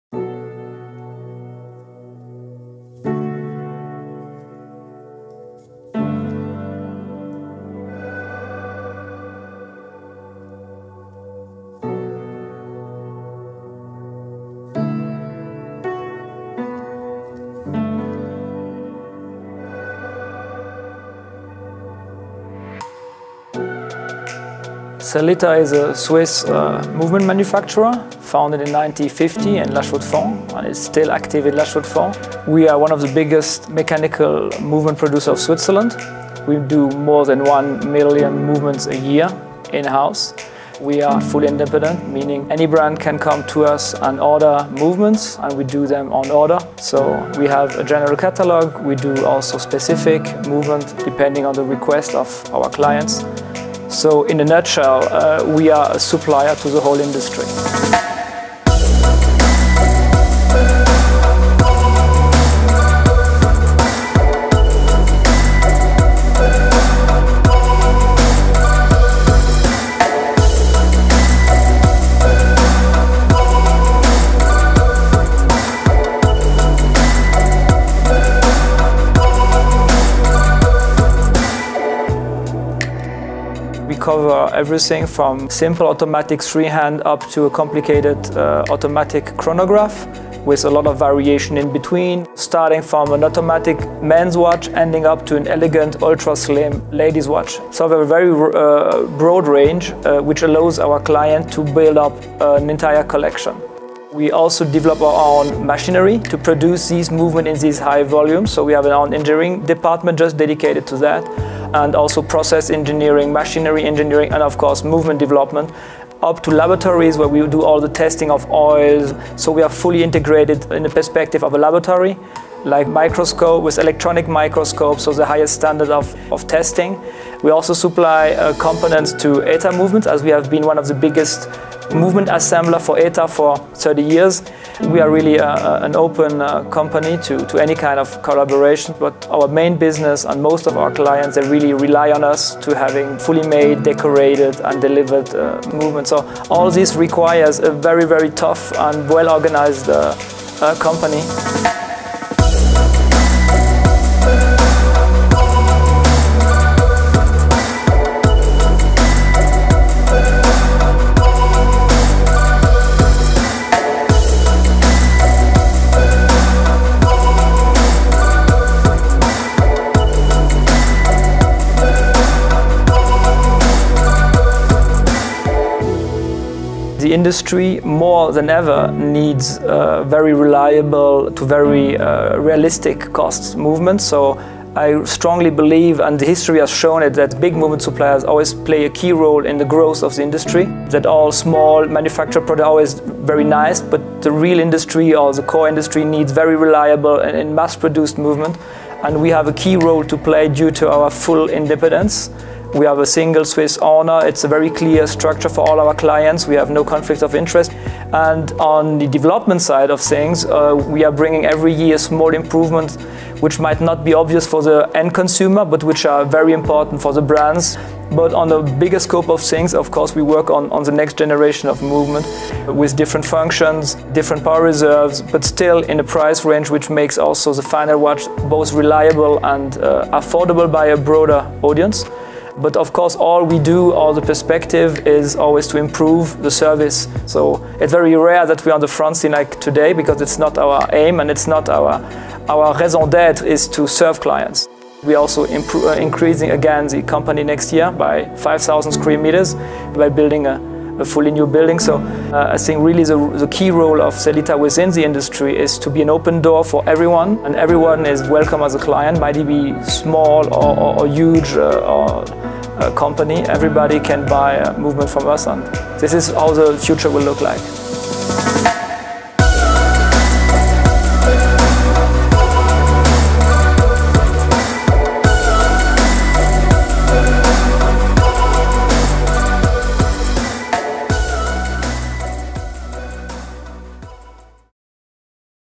Chrono24 Visits - Sellita | Swiss Watch Movement Manufacture
Chrono24 visits Sellita | Sellita is the second largest Swiss Made movement manufacturer after ETA. They are also 100% Swiss owned having one single owner who is the CEO of the company; Mr. Miguel Garcia. Chrono24 traveled to La-Chaux-de-Fonds to meet with Mr. Garcia and Mr. Sébastien Chaulmontet, Head of Innovation and Marketing. After our talks and a short interview, Mr. Chaulmontet walked us around the Sellita building. It is a huge and modern complex with state of the art technology, specialized ateliers and research department. From manufacturing parts to the final assembly, we saw every step of how a movement is made. Sellita. The brand has a rich past and a promising future, which is in safe hands with the current management. We wish them nothing but the best and thank them again for their openness and friendliness towards us during our visit.

If you want to sign up for our newsletter you can do so by clicking the